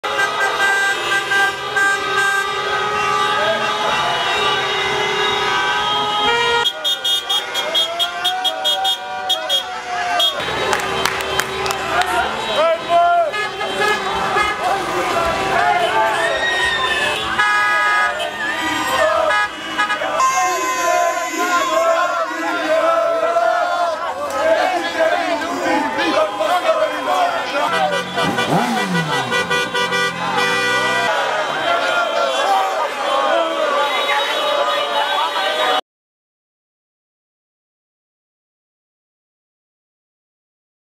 Slavlje na pulskim ulicama nakon pobjede Hrvatske nad Japanom
Pula, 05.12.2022 - Slavlje na gradskim ulicama nakon pobjede Hrvatske nad Japanom. Snimio: Goran Šebelić / CROPIX